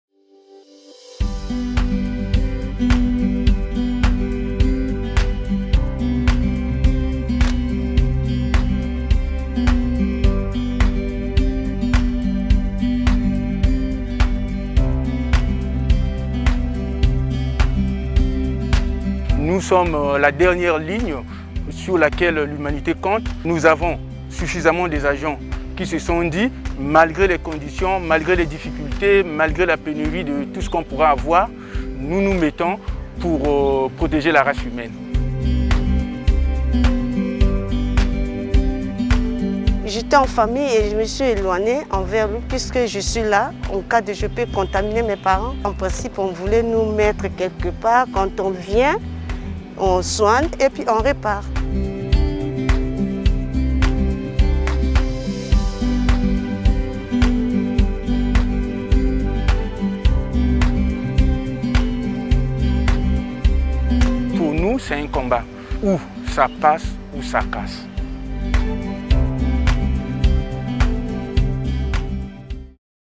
En première ligne de la bataille du Congo face au COVID-19
En République du Congo, les travailleurs de la santé travaillent 24 heures sur 24 en première ligne de la lutte contre le COVID-19. Malgré les risques quotidiens et les responsabilités croissantes, ils s'efforcent de traiter les patients et veiller à ce que les communautés soient protégées contre l'infection.